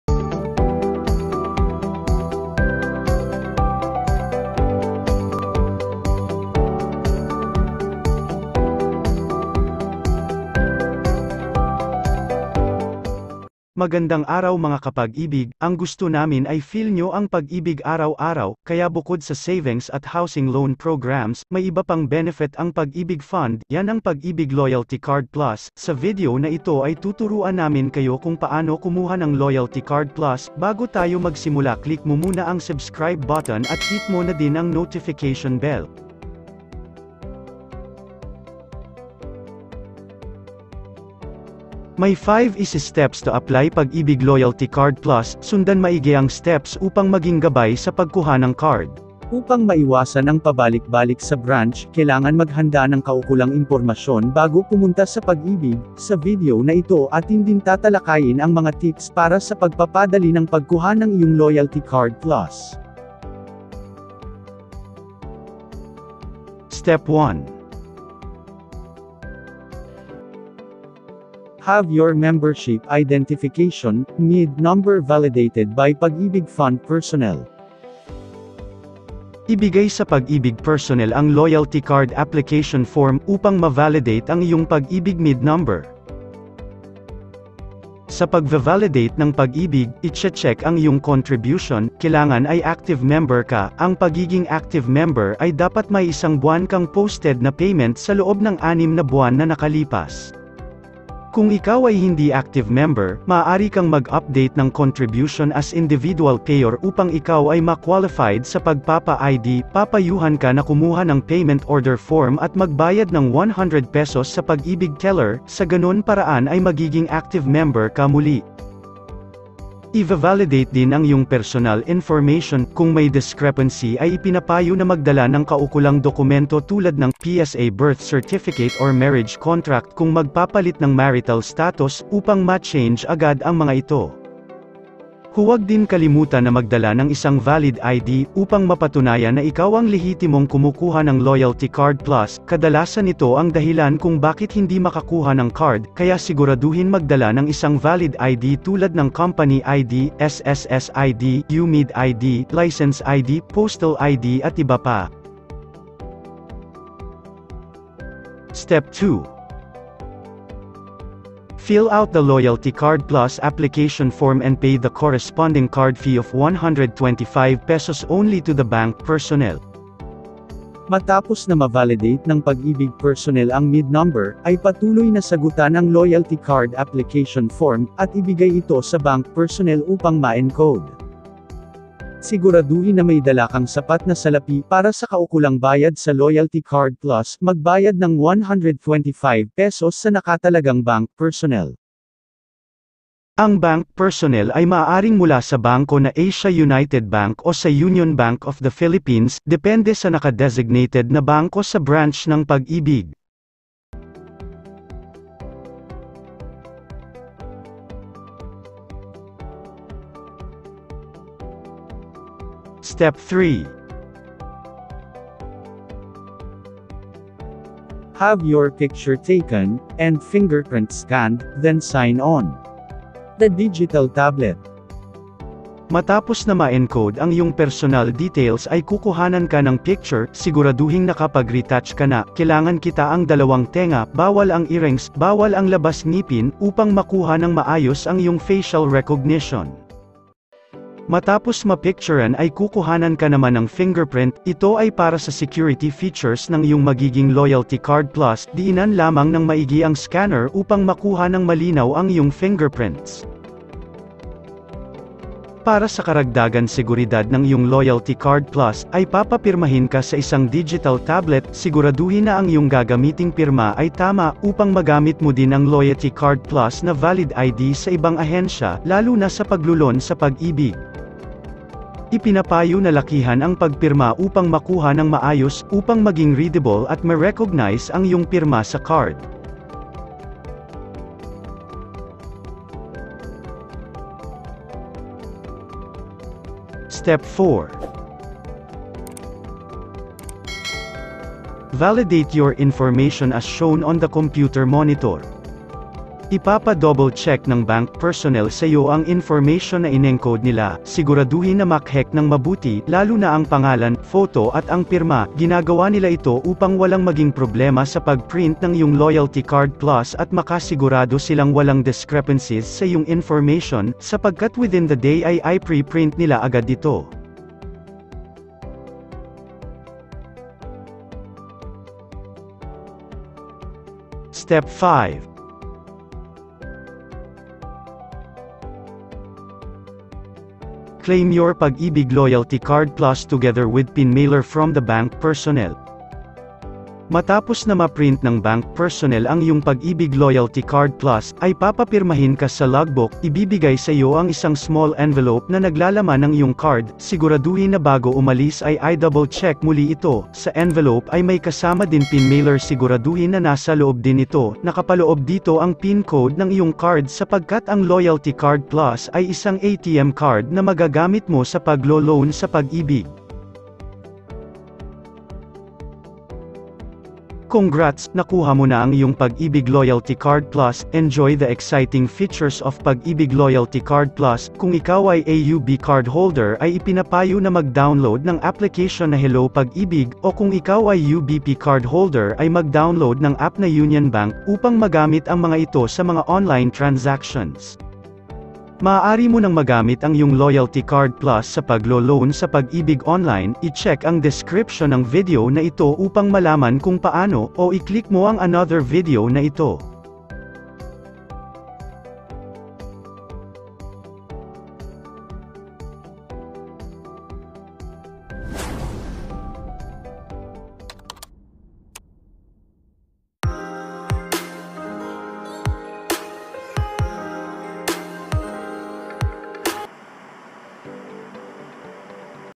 Pag-IBIG Loyalty Card Plus | How to Apply
The Pag-IBIG Loyalty Card Plus allows you to enjoy exclusive discounts and rewards on your grocery purchases, tuition fee,
hospital bills, fuel expenses, restaurant bills and many more from our more than 300 partner-establishments nationwide.
And now, you can also use it as a cash card where you may conveniently receive your Pag-IBIG
Multi-Purpose Loan (MPL) proceeds, MP2 Savings dividends, and other benefits.

Maari kayong mag file ONLINE . Please watch this video para ma iguide po kayo on HOW:

New Update! 2020 Paano makakapagloan sa Pag-IBIG -